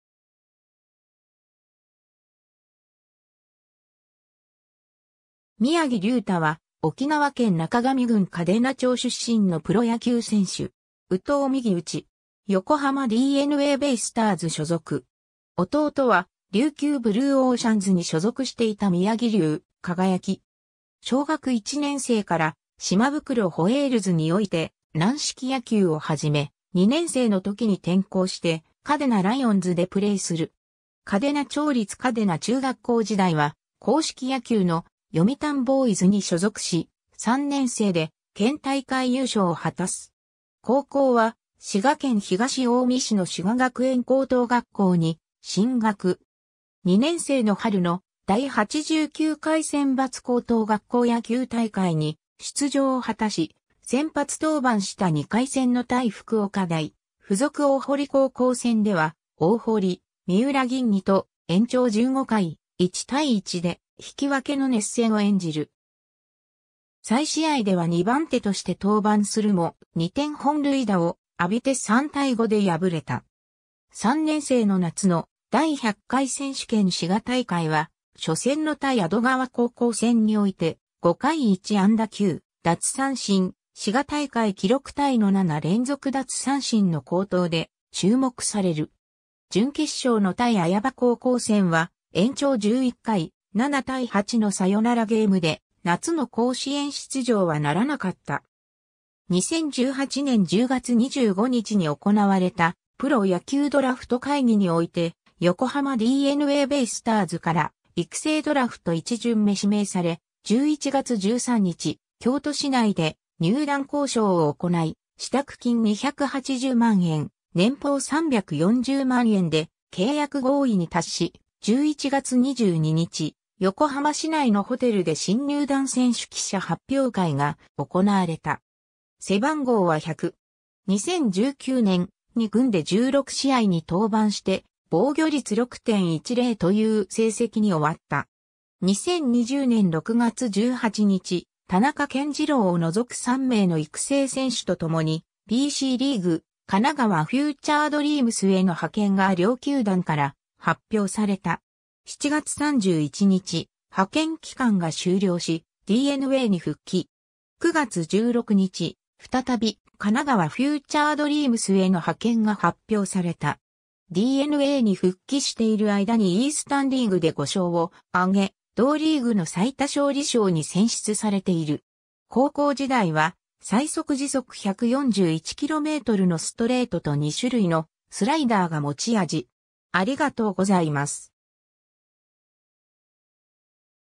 宮城滝太
宮城 滝太（みやぎ だいた、2000年7月15日 - ）は、沖縄県中頭郡嘉手納町出身のプロ野球選手（投手・育成選手）。
右投右打。
横浜DeNAベイスターズ所属。
弟は琉球ブルーオーシャンズに所属していた宮城竜輝。
 小学1年生から「島袋ホエールズ」において軟式野球を始め、2年生のときに転校して「嘉手納ライオンズ」でプレーする。
嘉手納町立嘉手納中学校時代は、硬式野球の「読谷ボーイズ」に所属し、3年生で県大会優勝を果たす。
 高校は滋賀県東近江市の滋賀学園高等学校に進学。
2年生の春の第89回選抜高等学校野球大会に出場を果たし、先発登板した2回戦の対福岡大付属大濠高校戦では、大濠・三浦銀二と延長15回、1対1で引き分けの熱戦を演じる。
再試合では2番手として登板するも、2点本塁打を浴びて3対5で敗れた。
3年生の夏の第100回選手権滋賀大会は、初戦の対安曇川高校戦において、5回1安打9奪三振、滋賀大会記録タイの7連続奪三振の好投で注目される。
準決勝の対綾羽高校戦は、延長11回、7対8のサヨナラゲームで夏の甲子園出場はならなかった。
 2018年10月25日に行われたプロ野球ドラフト会議において、横浜DeNAベイスターズから育成ドラフト一巡目指名され、11月13日、京都市内で入団交渉を行い、支度金280万円、年俸340万円(金額は推定)で契約合意に達し、11月22日、横浜市内のホテルで新入団選手記者発表会が行われた。
 2019年、二軍で16試合に登板して防御率6.10という成績に終わった。
 2020年6月18日、田中健二朗を除く3名の育成選手とともにBCリーグ・神奈川フューチャードリームスへの派遣が両球団から発表された。
7月31日、派遣期間が終了しDeNAに復帰。
9月16日、再び神奈川フューチャードリームスへの派遣が発表された。
DeNAに復帰している間にイースタン・リーグで5勝をあげ、同リーグの最多勝利賞に選出されている。
 高校時代は最速141km/hのストレートと2種類のスライダーが持ち味。